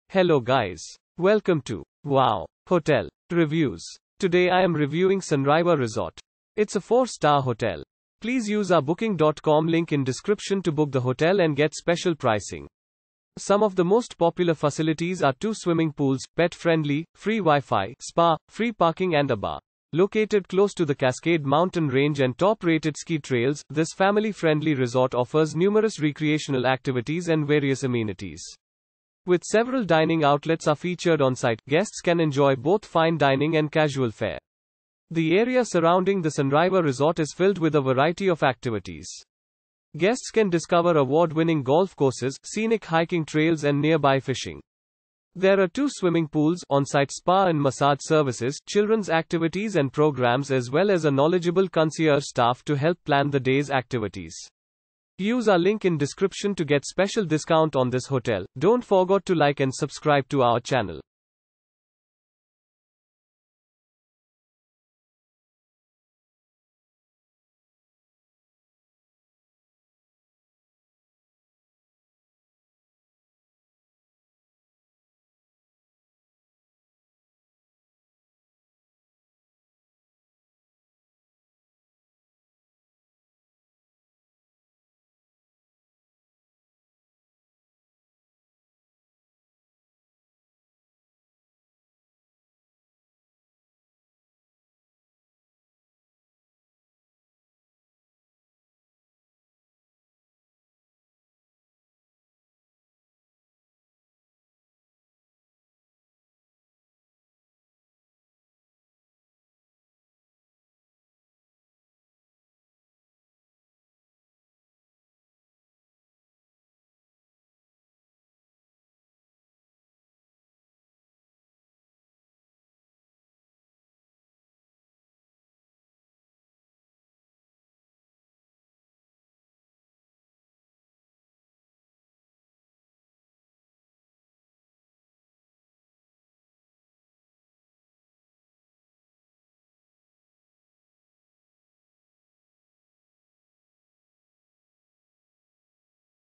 Sunriver Resort Review - Sunriver , United States of America
Sunriver Resort

Located close to the Cascade Mountain Range and top-rated ski trails, this family-friendly resort offers numerous recreational activities and various amenities. With several dining outlets are featured on-site, guests can enjoy both fine dining and casual fare.
The area surrounding the Sunriver Resort is filled with a variety of activities. Guests can discover award-winning golf courses, scenic hiking trails and nearby fishing.
There are 2 swimming pools, on-site spa and massage services, children's activities and programs as well as a knowledgeable concierge staff to help plan the day's activities.